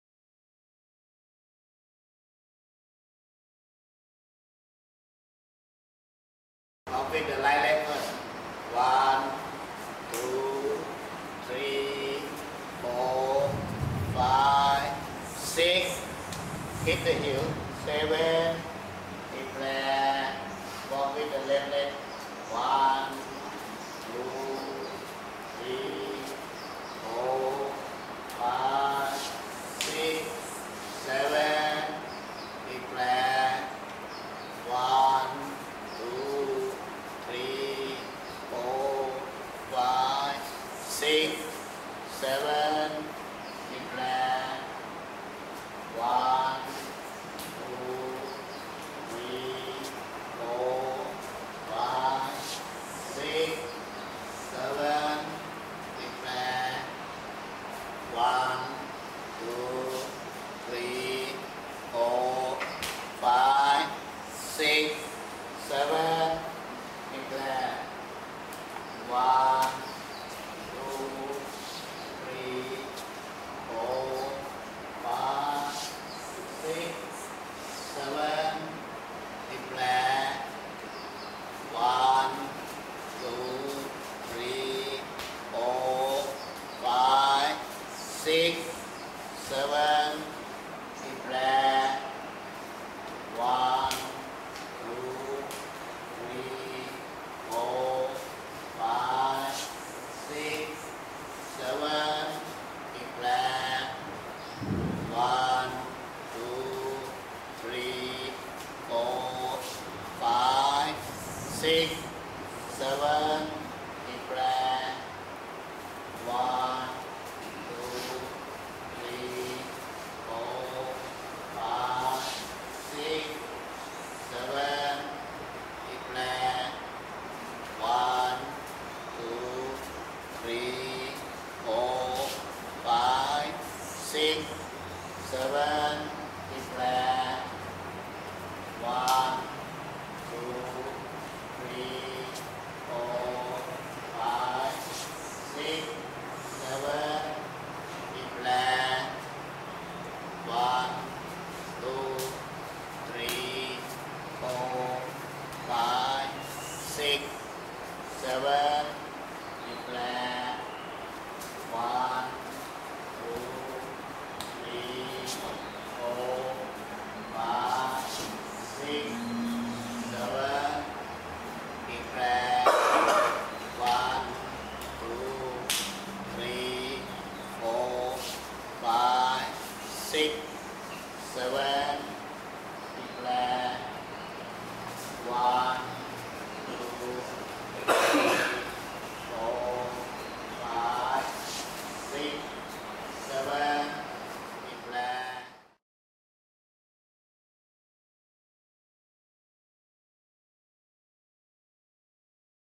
Ajahn Suthep: 7 Step Walking Meditation
A short video with Ajahn Suthep demonstrating 7-step lotus walking meditation. Recorded at Bodhi Lankarama Buddhist Temple, August 30th, 2009, as part of the 120th Anniversary Celebrations.